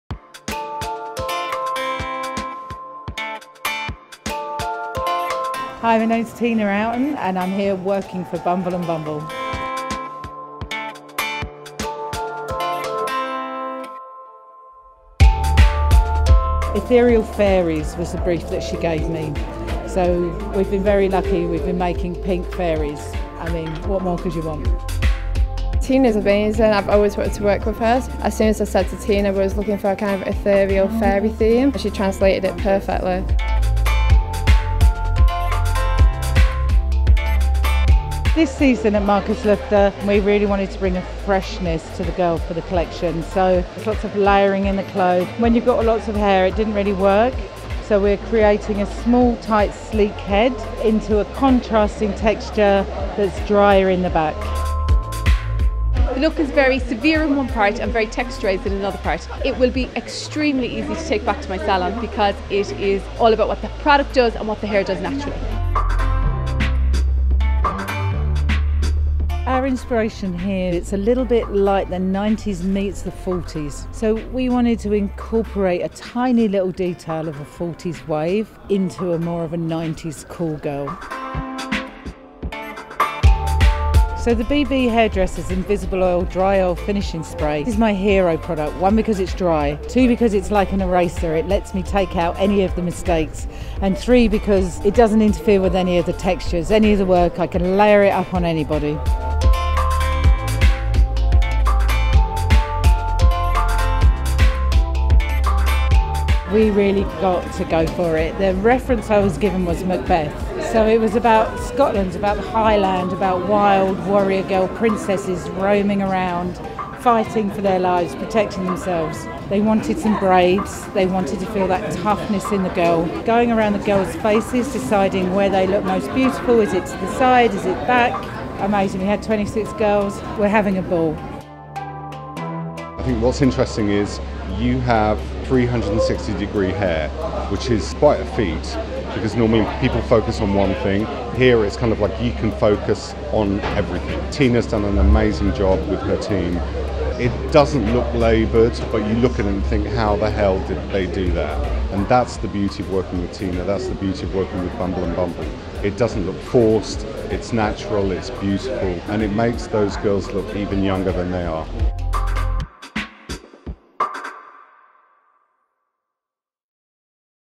Sobe Brown London Fashion Week 2017
We had the great privilege of working with the amazing Tina Outen & Bumble & Bumble again for London Fashion Week 2017. Here is some behind the scenes of some of the creations over the few days. Thank you again to Tina & Bumble for the use of the video.